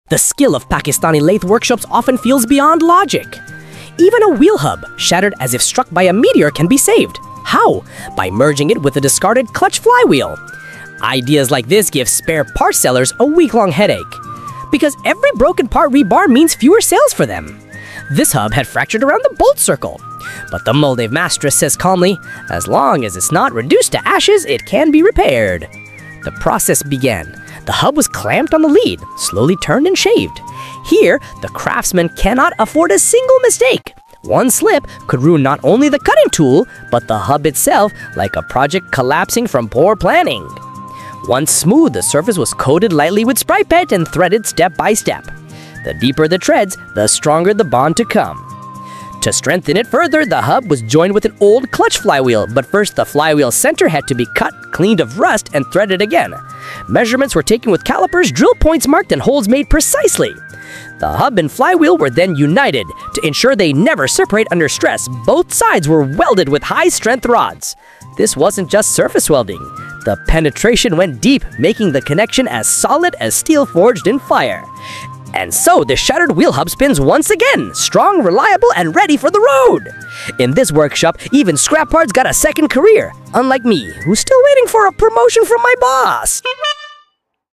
Broken Wheel Hub Repaired
From scrap metal to a working hub again, this process shows the unmatched creativity and craftsmanship of Pakistani workshops.